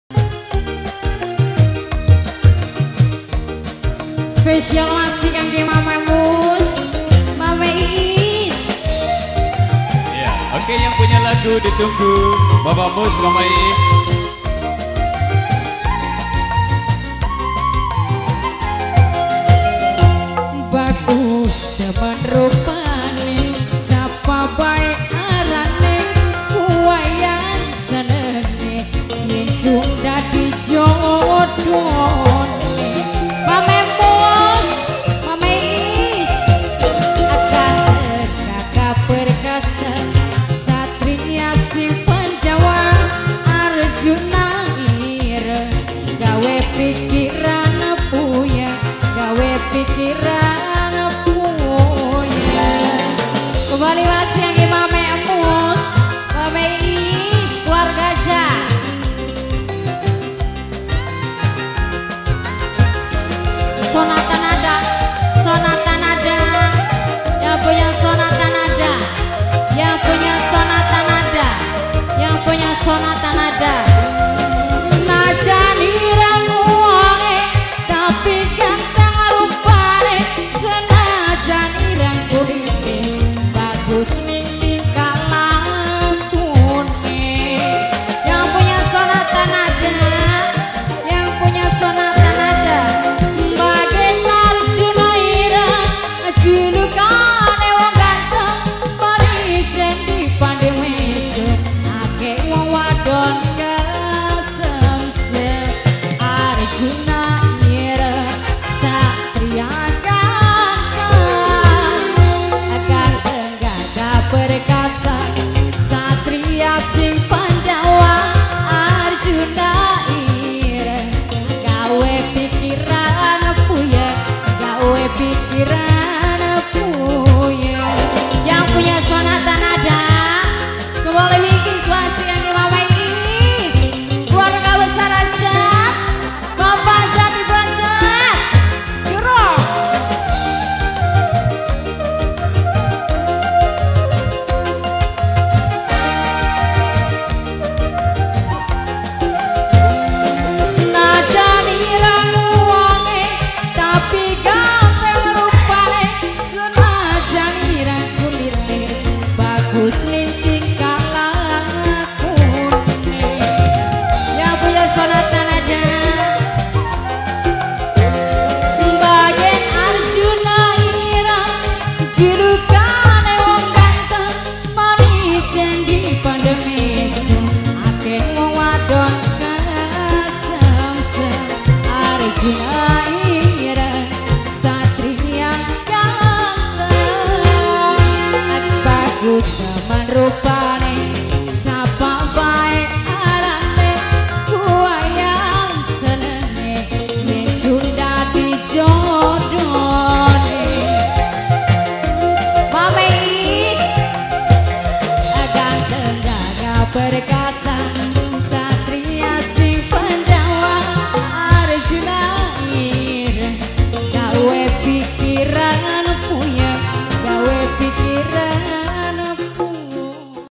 ARJUNA IRENG - BUROK MA NADA-JAPURA LOR-BLOK.PAHING-PANGENAN-CIREBON KAMIS 14,JULI 2022
KHITANAN. TIO FEBRIAN
PEMANGKU HAJAT.BPK. TOTO & IBU. IPA
DESA.JAPURA LOR - BLOK. PAHING
KEC.PANGENAN - KAB.CIREBON
HARI.KAMIS,14 JULI 2022